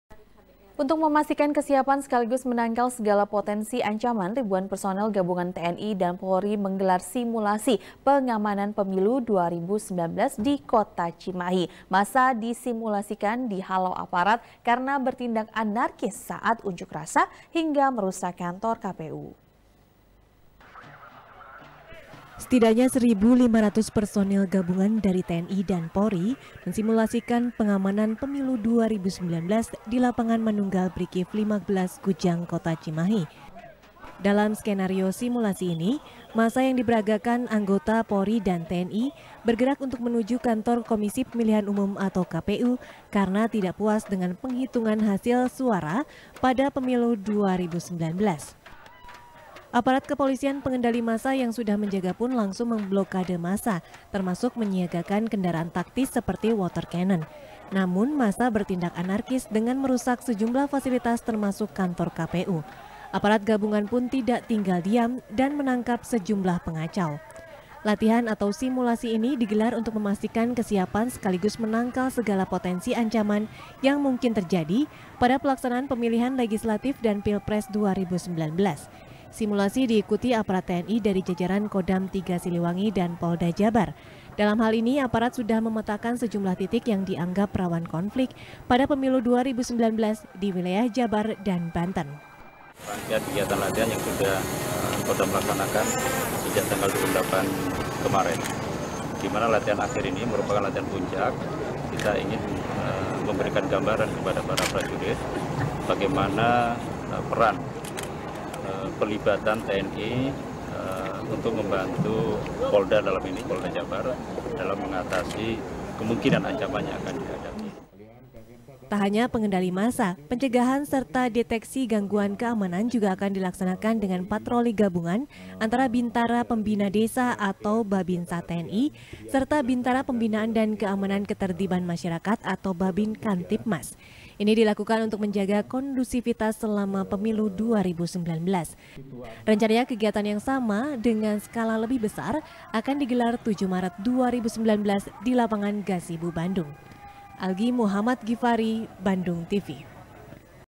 Tni Polri Gelar Simulasi Pengamanan Pemilu 2019 | SBR | BANDUNGTV
| SBR | BANDUNGTV | KRISANTY | Untuk memastikan kesiapan sekaligus menangkal segala potensi ancaman, ribuan personel gabungan tni dan polri menggelar simulasi pengamanan pemilu 2019 di kota cimahi. massa disimulasikan dihalau aparat, karena bertindak anarkis saat unjuk rasa, hingga merusak kantor kpu.